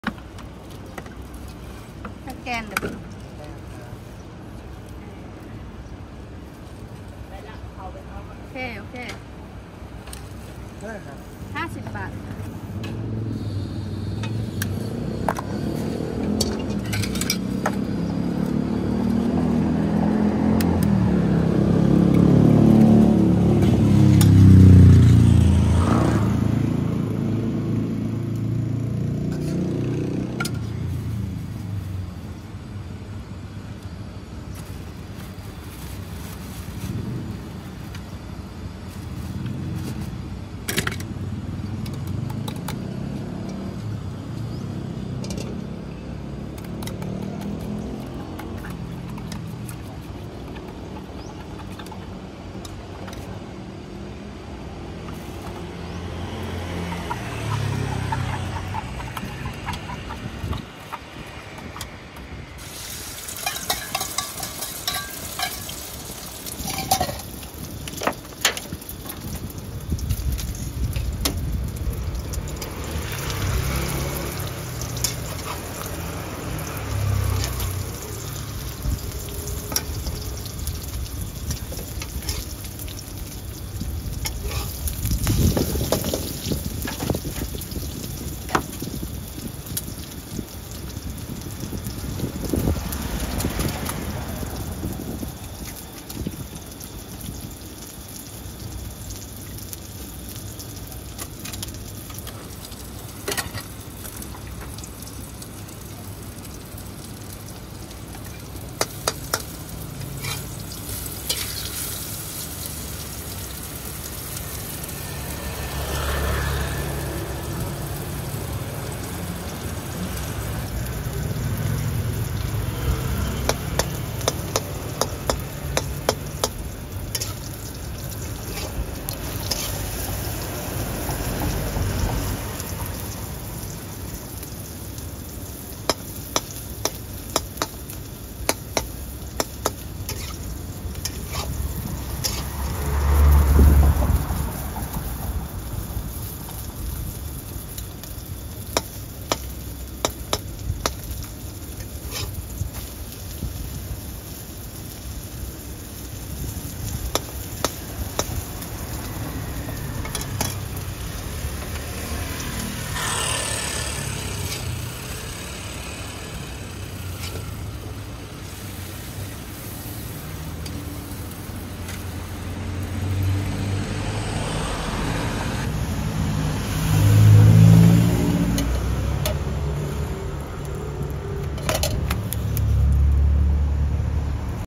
Khai Jiao, The Best Lunch Food in Thailand?
Business Hours: Everyday
Price: 50 THB / 1.45 USD
Address: Sukhumvit Soi 61, Bangkok